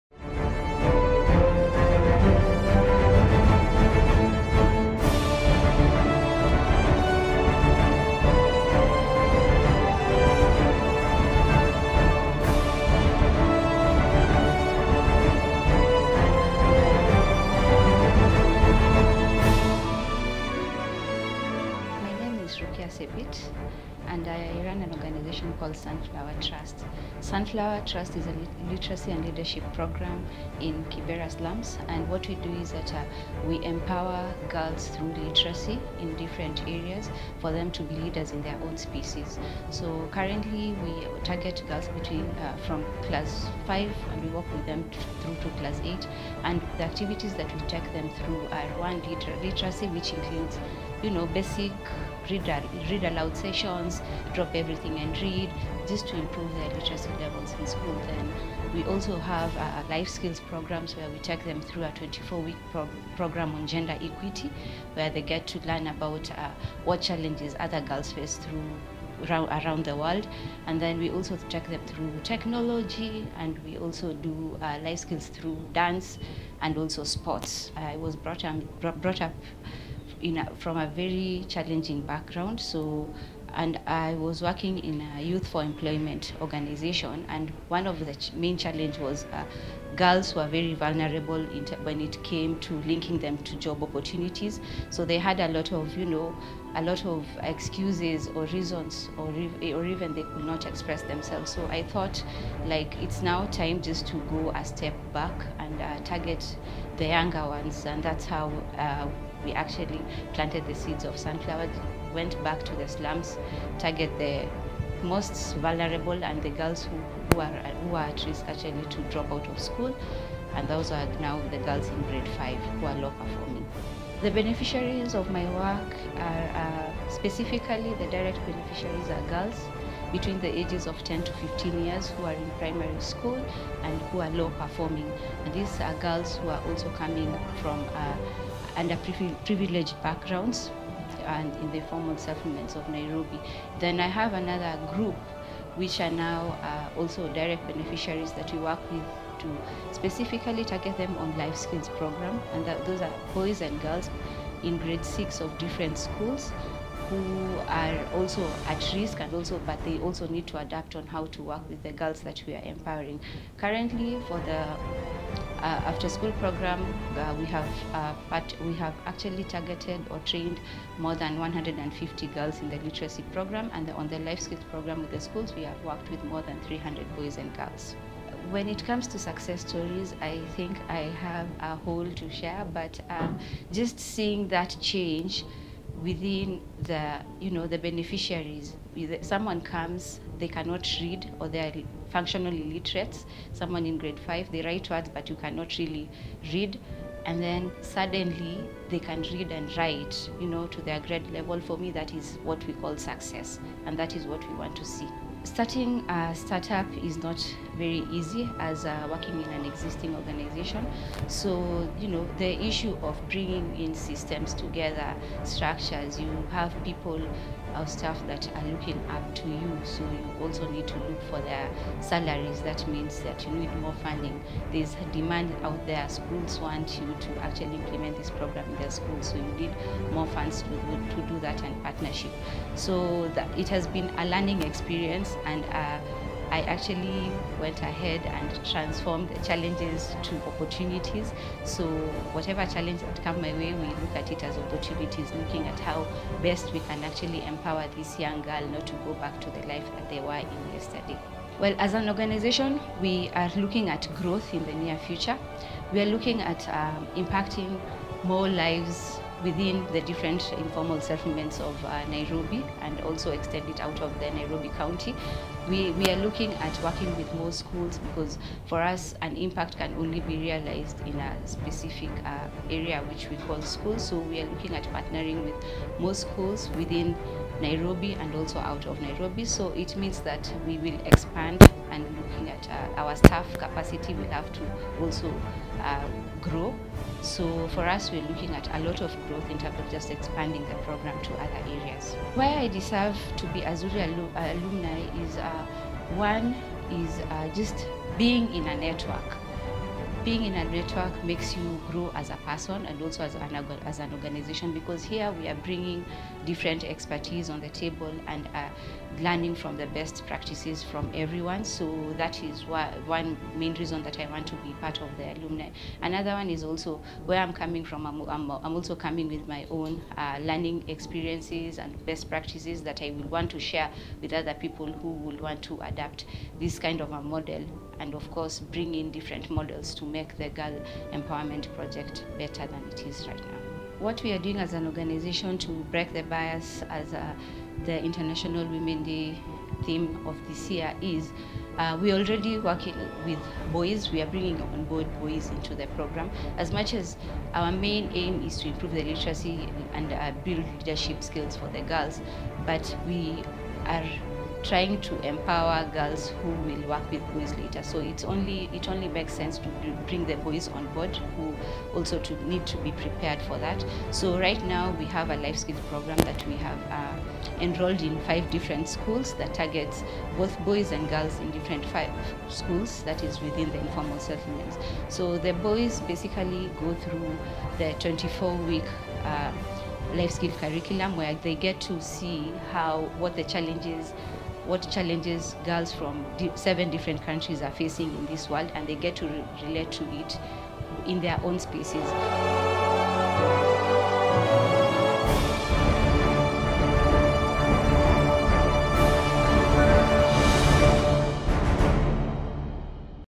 Rukia Sebit - Education Honoree
Rukia Sebit is the founder of The Sunflower Trust and a ZuriAwards 2022 Nominee. The Sunflower Trust exists to create an environment where adolescent girls can discover as a community, their capacities and potential, achieve self-value, self-actualization and acquiring bodily autonomy and knowledge to make informed choices as measured by health seeking behavior, access to quality education and economic security. Give targeted group an access to quality learning that
will reduce their risk of dropping out of school.